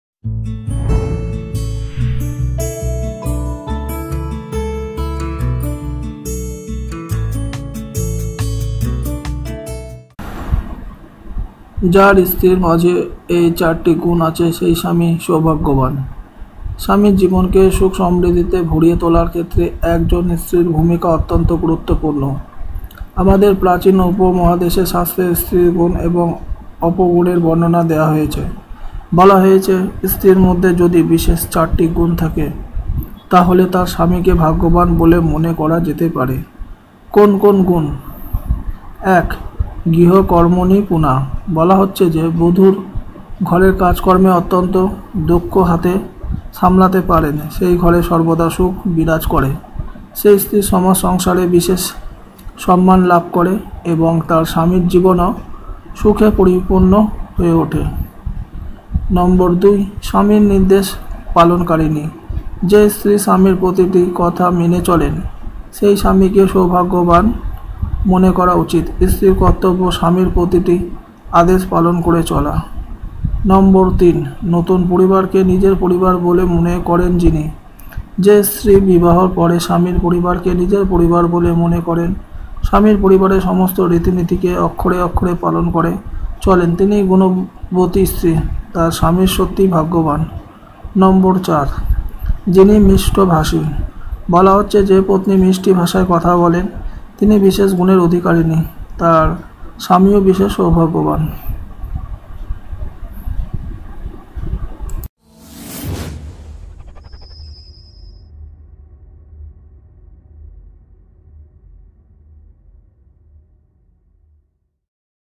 Impotent tips_If there are four qualities in the wife that is good.
জীবনকে সুখসমৃদ্ধিতে ভরিয়ে তোলার ক্ষেত্রে এক জন স্ত্রীর ভূমিকা অত্যন্ত গু‌রুত্বপূর্ণ। প্রাচীন ভারতীয় শাস্ত্রে স্ত্রীর গুণ এবং অপগুণের বর্ণনা দেওয়া হয়েছে। বলা হয়েছে, স্ত্রীর মধ্যে যদি বিশেষ চারটি গুণ থাকে, তা হলে তার স্বামীকে ভাগ্যবান বলে মনে করা যেতে পারে। কোন কোন গুণ? আসুন, জেনে নিই-১. গৃহকর্মনিপুণা: 'গরুড়পুরাণে' বলা হচ্ছে, যে বধূ ঘরের কাজকর্মে অত্যন্ত দক্ষ হাতে সামলাতে পারেন, সেই ঘরে সর্বদা সুখ বিরাজ করে। সেই স্ত্রী সমাজ-সংসারে বিশেষ সম্মান লাভ.